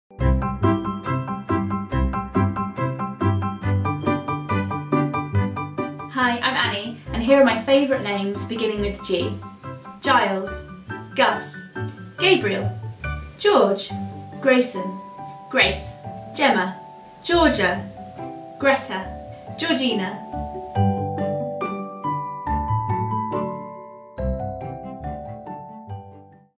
Most popular baby names with the letter G I Emma's Baby Name Zone
Mum, Annie is sharing her most favourite baby names beginning with the letter G. Varieties include both baby girl and boy names as well as those that are considered unusual and unique baby names. Some of these choices are the most popular baby names out there - would you add any of these to your list?